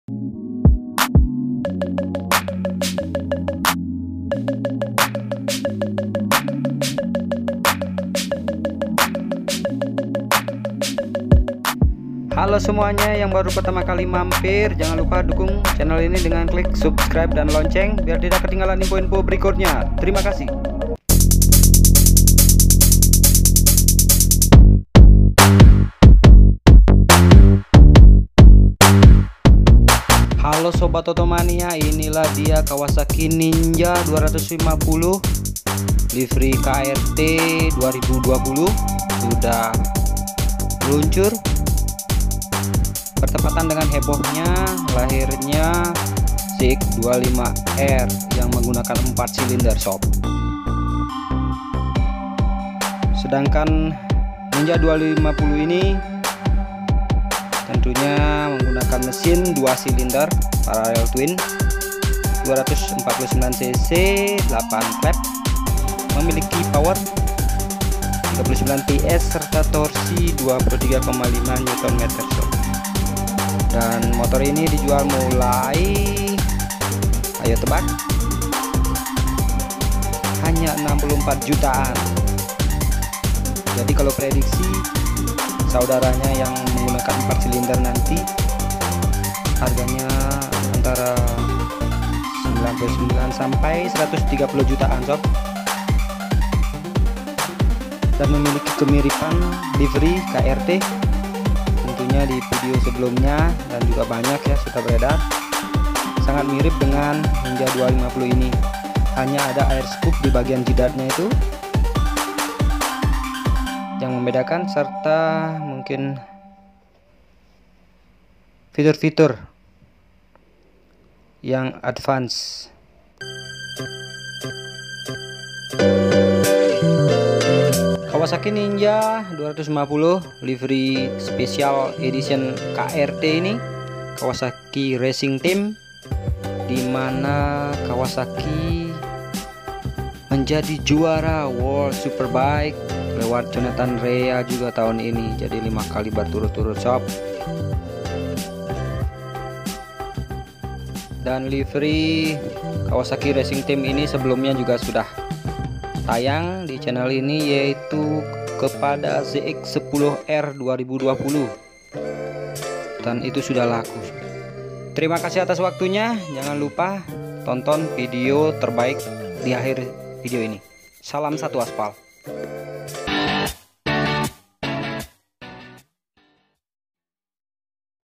New KAWASAKI NINJA 250 2020 KRT edition | First impression walkaround
Livery KRT, Kawasaki Racing Team 2020 pada Ninja 250 terbaru. Dihargai mulai 64 jutaan,Kawasaki ninja 250 krt livery 2020 sangat menarik. Livery ini jadi sangat spesial bukan hanya karena kemenangan jonathan rea di wsbk untuk yang kelima kalinya tetapi juga launching kawasaki zx25r yang memakai livery krt ini.